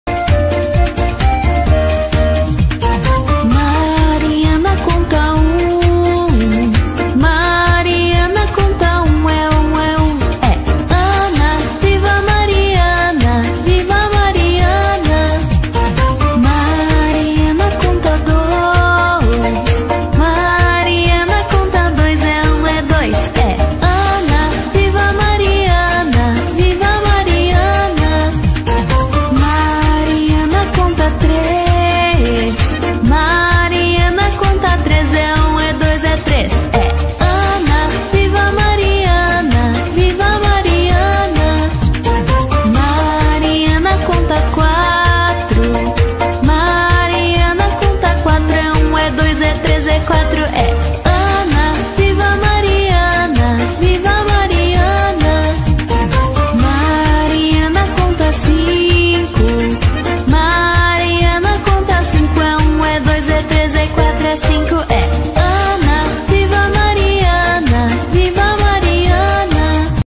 MARIANA CONTA 1 ao 5 com a turma da Galinha Pintadinha/Aprender a contar/Educativo
A Mariana conta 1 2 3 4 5 com a turma da Galinha Pintadinha. Vídeo para Educação Infantil

Olá bellos e bellas, segue mais um  vídeo para ajudar no aprendizado de vocês.

Canal administrado e monitorado pela responsável.

36canaldamaribella